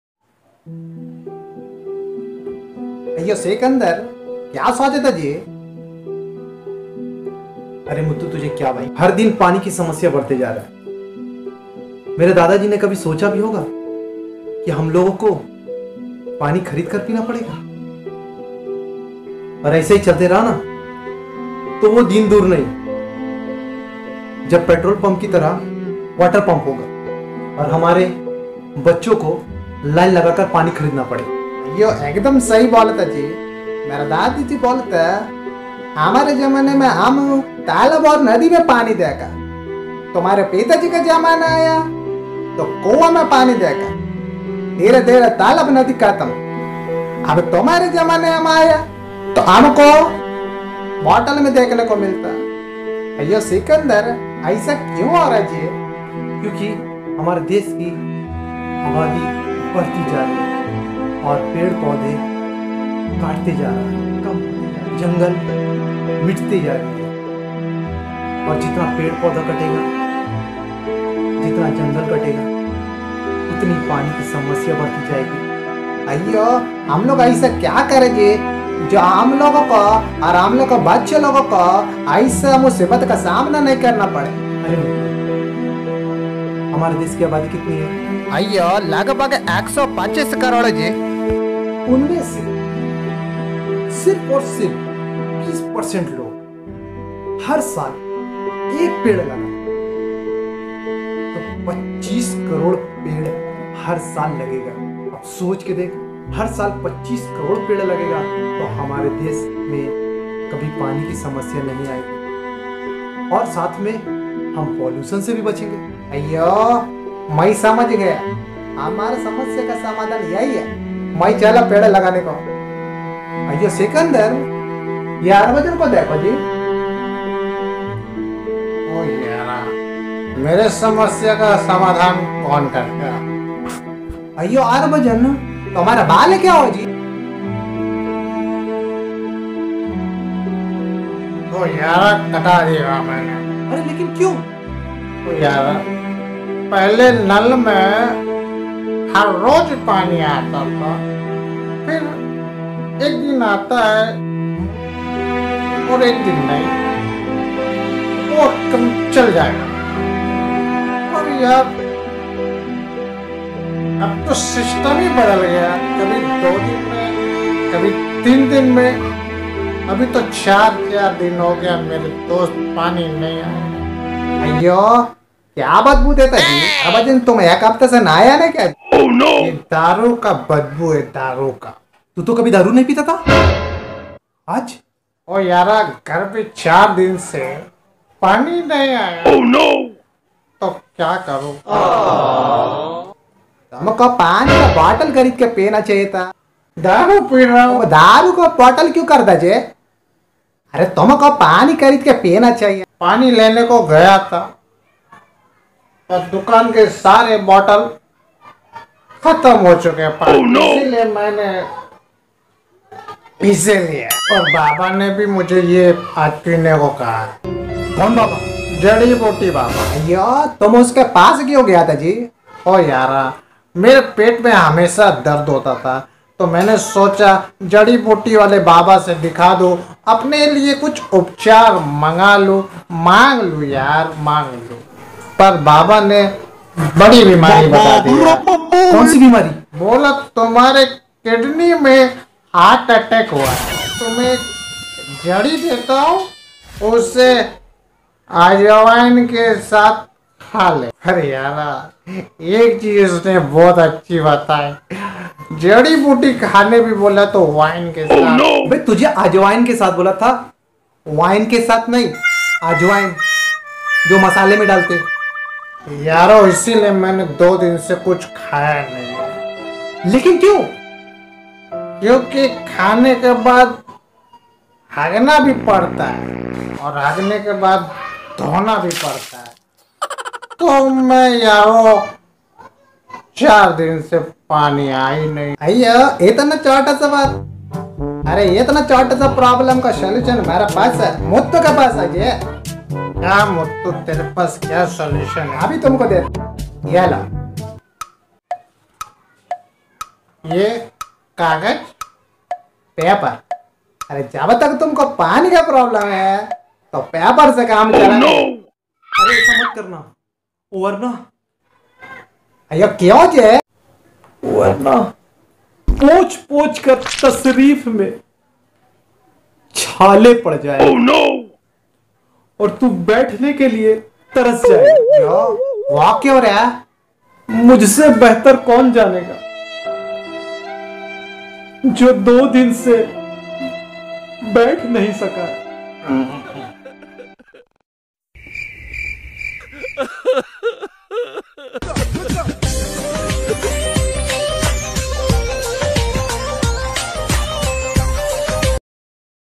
| Paani ki Samasya | | water problem | | save water save life | | SUPERHIT TVR |
Dosto aaj har din pani ki samasya badti jarahi hai. To aap log kosis Kare paid paudha laganeka. Or is video ke jarye kosis kiya hun hasaneka or aap tak kuchh messages pahuchaneka.